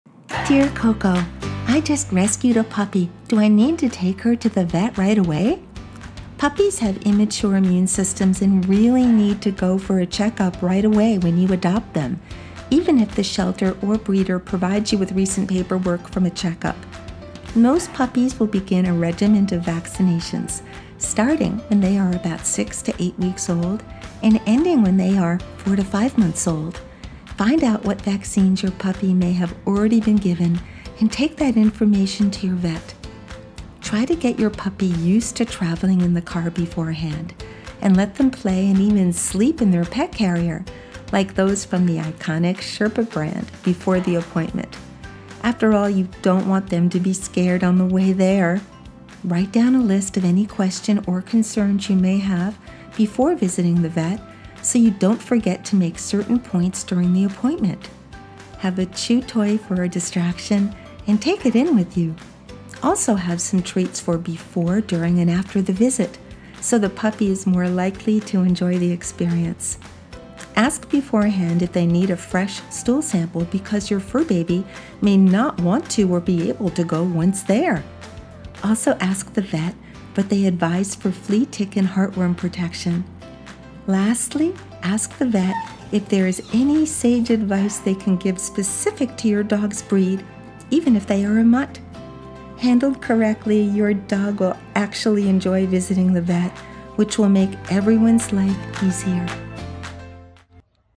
Ask KoKo - Puppy's first visit to the vet. KoKo explains how to prepare for your first Vet visit.
Even if your new puppy came with papers indicating a recent Vet visit, you should not put off going in yourself right away. KoKo explains how to prepare for your fur baby's initial Vet experience.